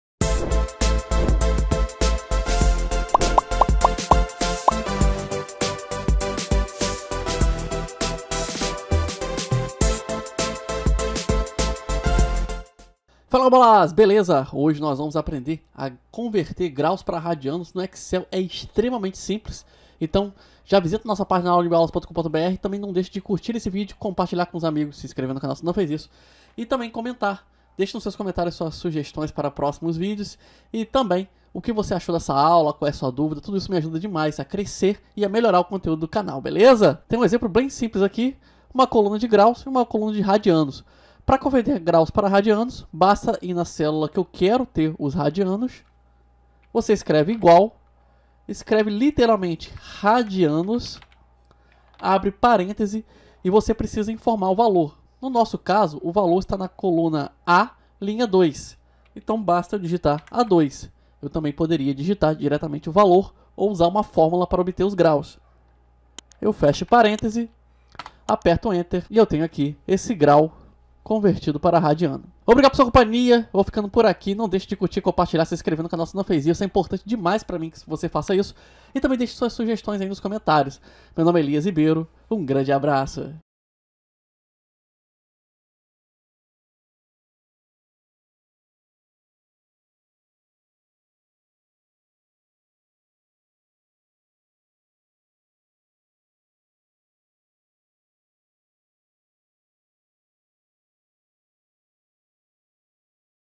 📊 EXCEL RADIANOS: Aprenda a converter Graus para Radianos no Excel, de forma fácil e rápida.
Partes:
00:00:00 Vinheta
00:00:13 Explicação e introdução
00:00:36 Converter Graus em Radianos
00:01:12 Considerações finais
00:01:23 Tela final

Assista também: "🤖 .net MAUI Aula 27 - Picker: Adicionando Itens por XAML"